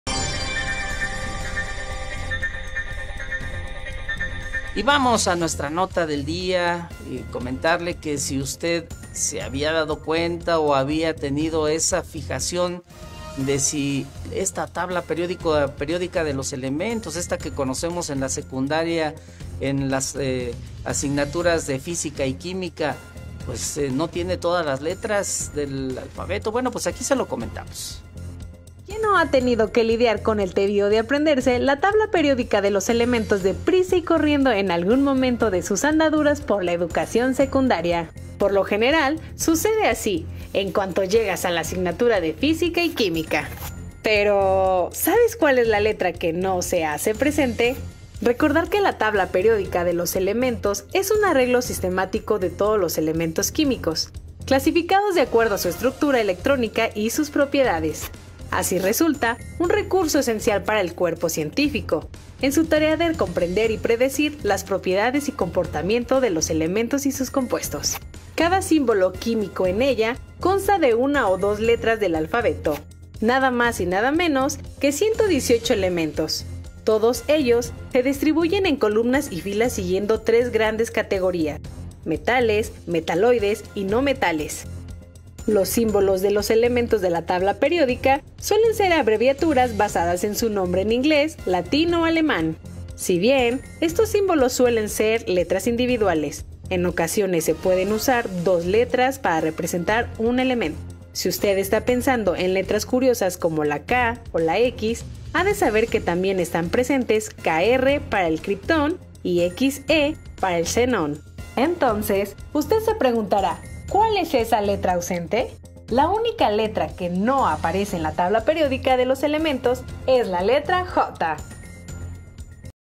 ué letra del alfabeto no ha sido utilizada en la tabla periódica de elementos químicos
Quién no ha tenido que lidiar con el tedio de aprenderse la tabla periódica de los elementos de prisa y corriendo en algún momento de sus andaduras por la educación secundaria.